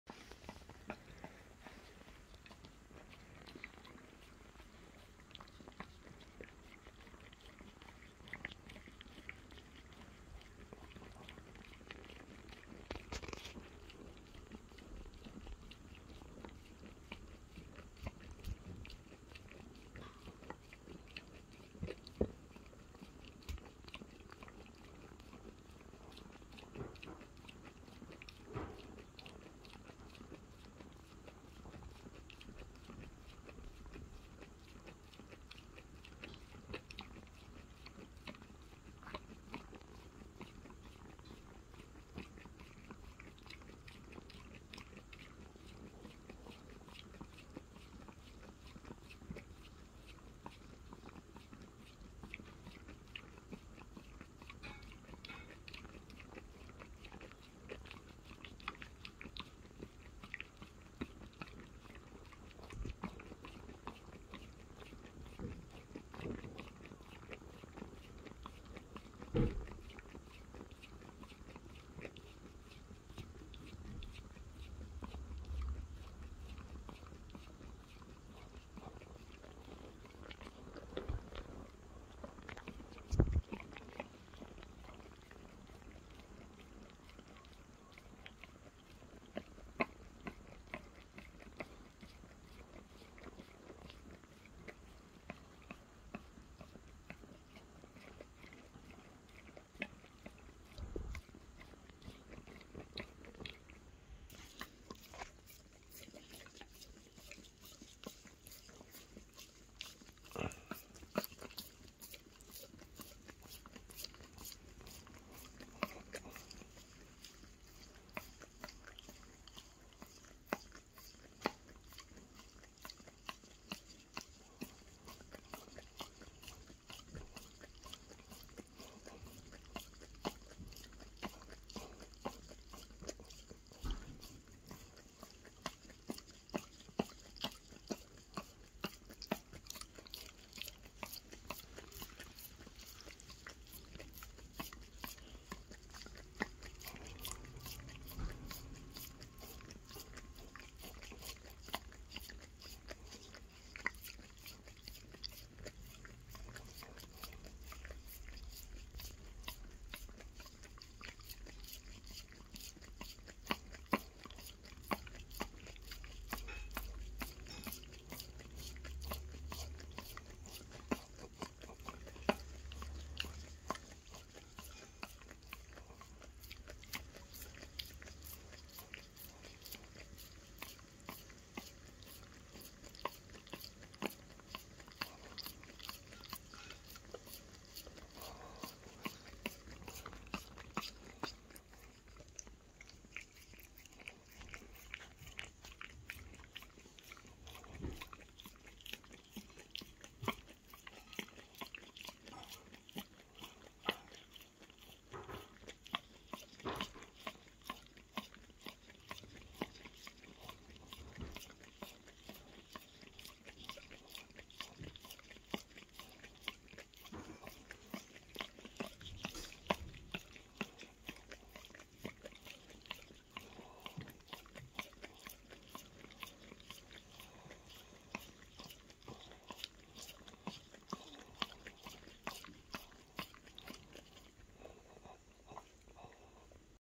Rough Collie Clary's Christmas Froyo ASMR!
Clary's Christmas Froyo ASMR!
Here is a longer version of my rough collie with her Christmas froyo. Look for the card in the video of how to make this Christmas froyo for your dog(s). Merry Christmas and happy new year!

Interested as a content creator in a tool that will help you grow? Try TubeBuddy!